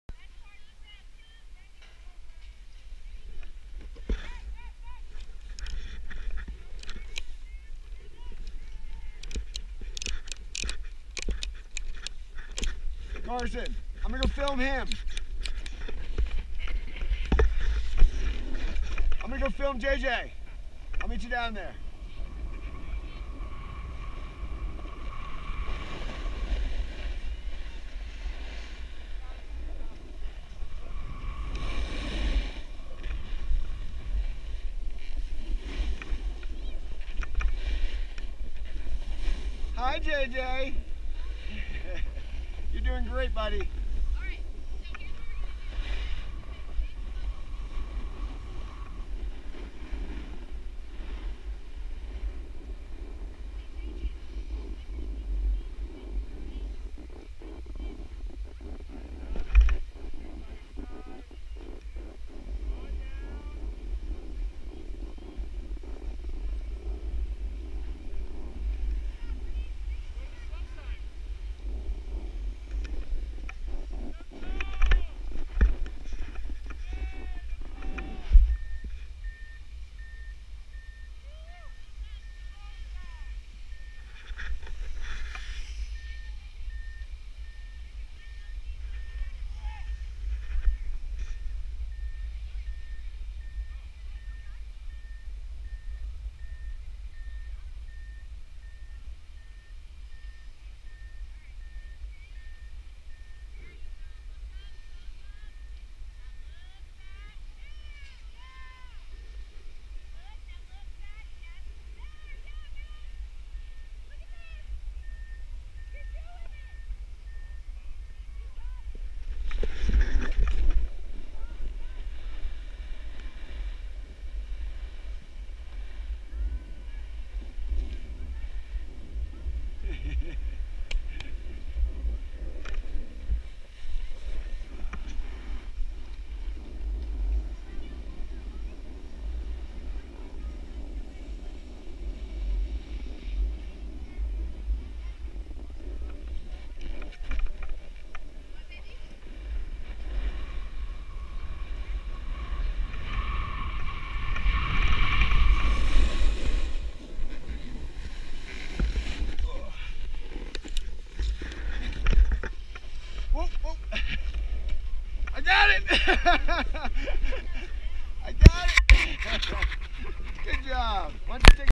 Carson and Jevin learning to Snowboard at Wolf Creek